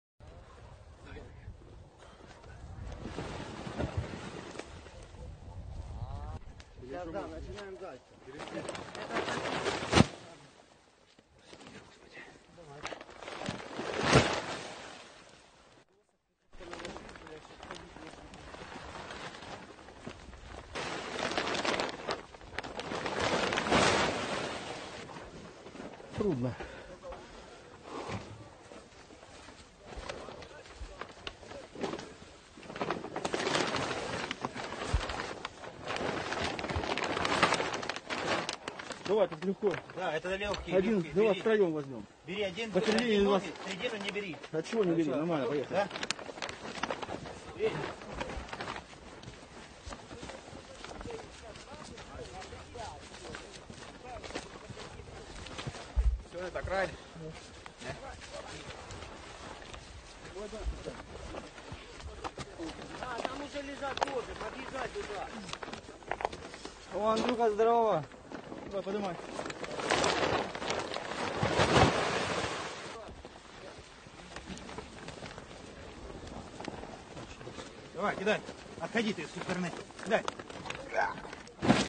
ویدئویی از دفن اجساد در گورهای دسته جمعی پس از بمباران شهر بیش از دو هفته توسط ارتش متجاوز
دفتر شهردار ماریوپول اوکراین گزارش کرد که طی ۱۲ روز محاصره و گلوله باران این شهر ۱۵۸۲ غیرنظامی کشته شده‌اند.